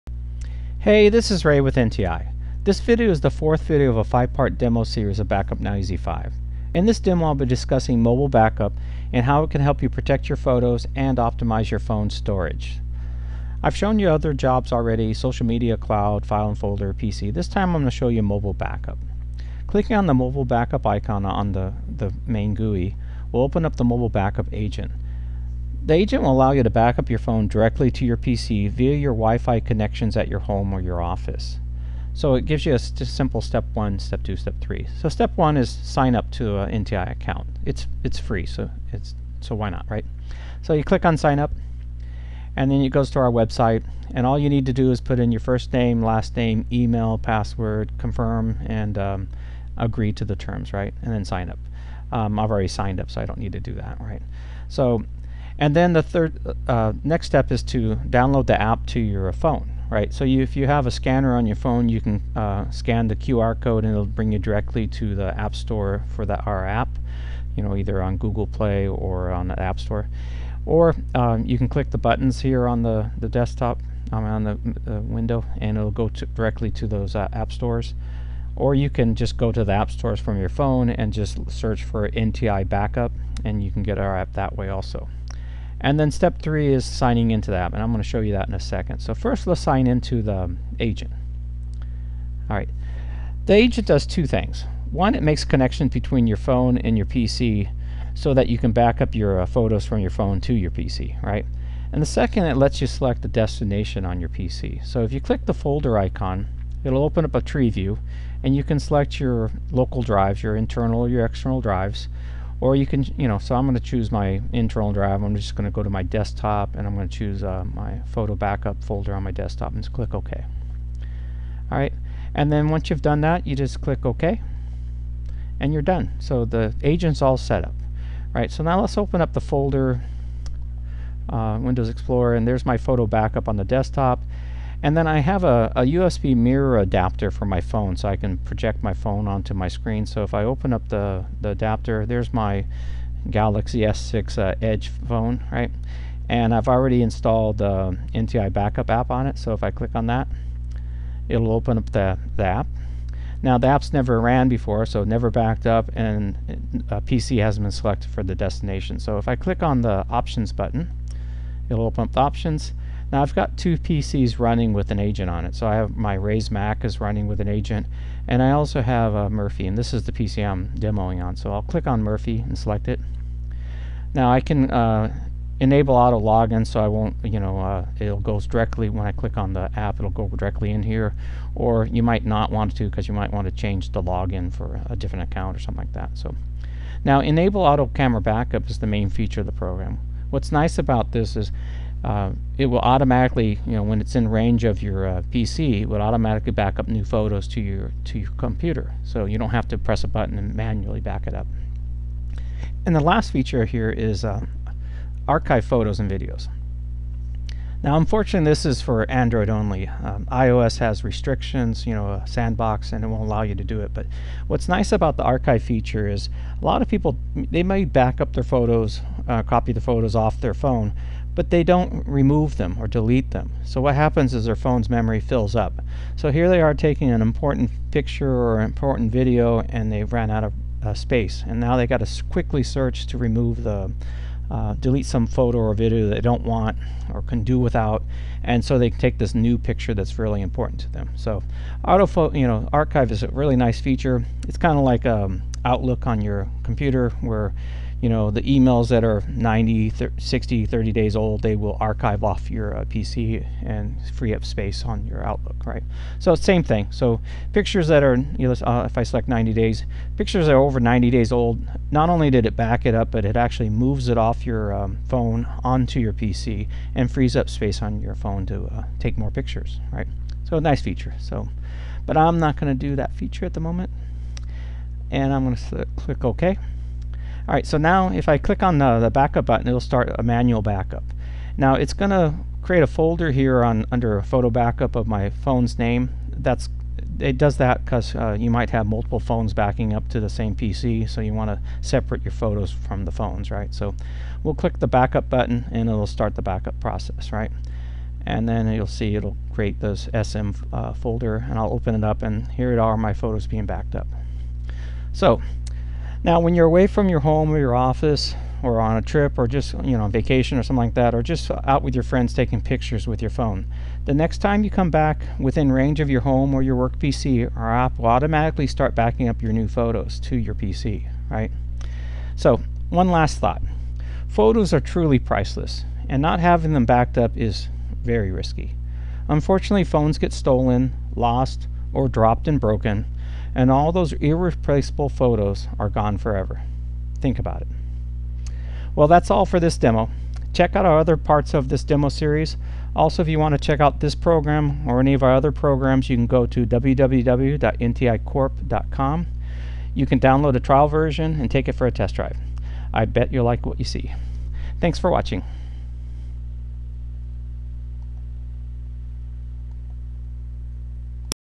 Backup Now EZ 5 - Mobile Backup Part 4 of 5
This is a demo video of Backup Now EZ 5 that highlights the Mobile Backup function.